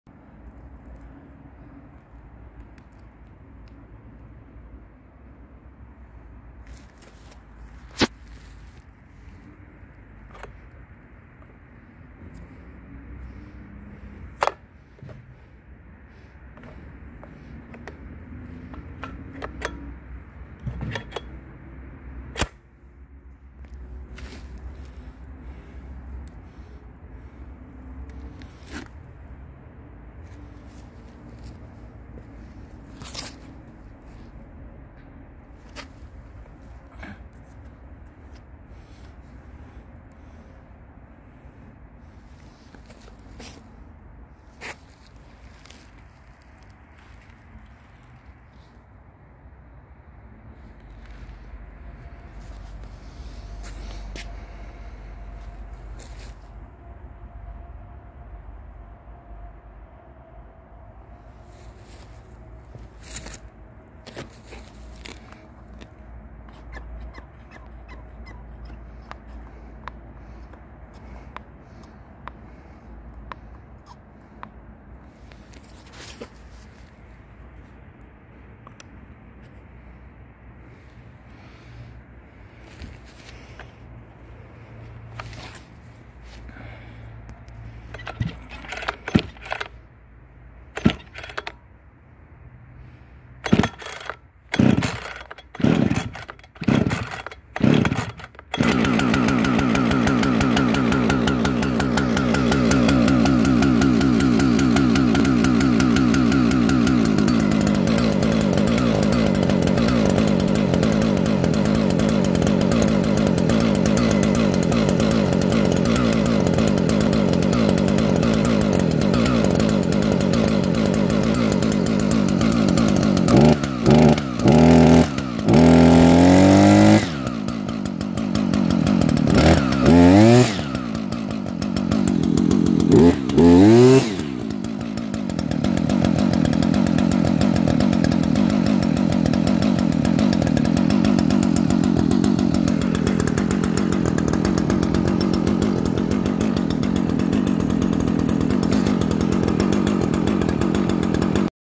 Husqvarna 545rxt AutoTune
2018 Husqvarna 545rxt AutoTune, tecomec trimmer head, 2.7mm scorpion desert line, soft start handle upgrade